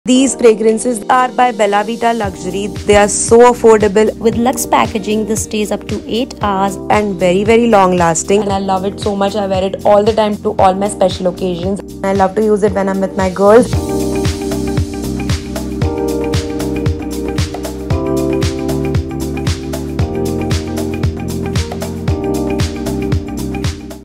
Grab Your Hands on Bella Vita Luxury Perfumes
Looking for a unisex perfume that lasts all day, making you feel super fresh? 🌊

Stop your search with the Bella Vita Luxury Fresh Unisex Perfume . With Almond and Bergamot notes at the top, 🌸Lavendar at its heart and Ylang-ylang at its base, start your day with the best perfume that makes you feel energetic while you take on the day. 🏃🏻‍♀️

Key benefits:
✔️Long-lasting
✔️Gender-neutral scent
✔️Eau De Toilette